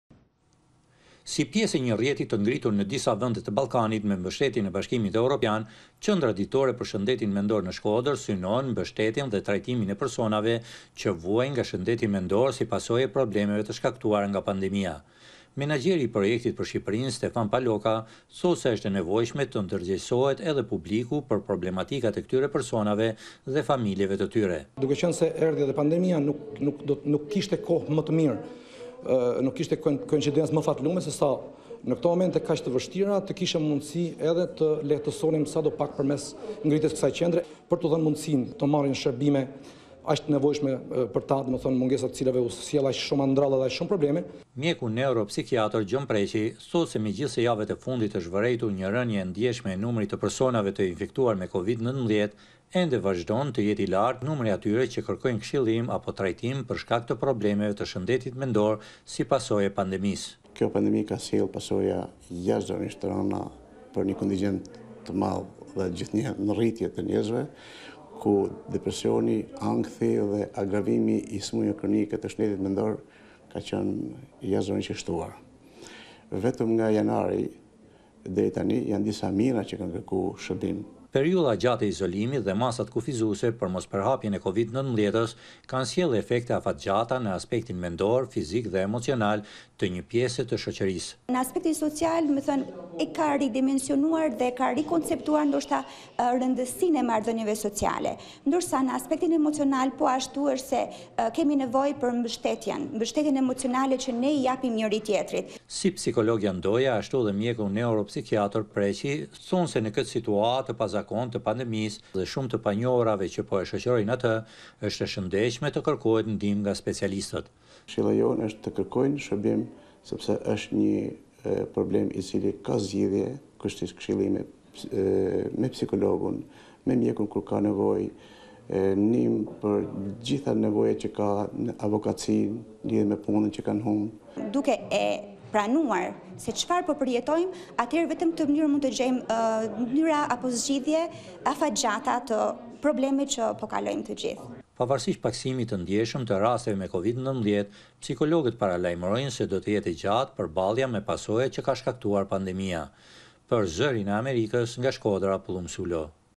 Pandemia dhe pasojat në shëndetin mendor
Megjithë uljen e ndjeshme të rasteve të infektimit me COVID-19, ekspertët thonë se periudha e gjatë e pandemisë vazhdon të shoqërohet me pasoja edhe për shëndetin mendor të një pjese të popullatës. Për t’u ardhur në ndihmë këtyre përsonave, në Shkodër u hap sot Qendra Ditore për Shëndetin Mendor “Unë Kujdesem për Ty”, pjesë e një rrjeti qendrash të ngjashme, të ngritura në rajonin e Ballkanit me mbështetjen e Bashkimit Europian.